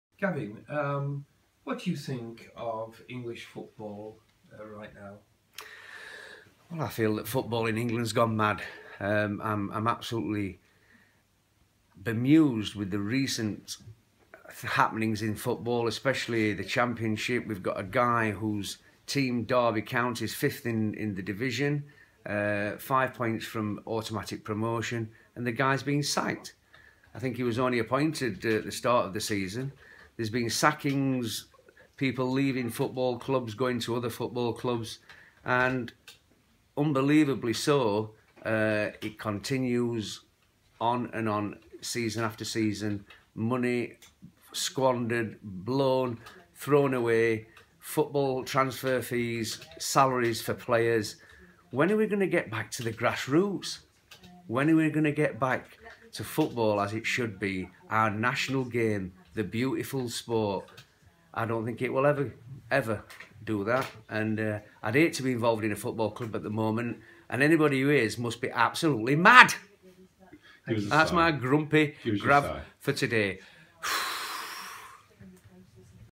Grumpy Gav Talks English Football
In this episode Gave bemoans the state of British Football... Sigh.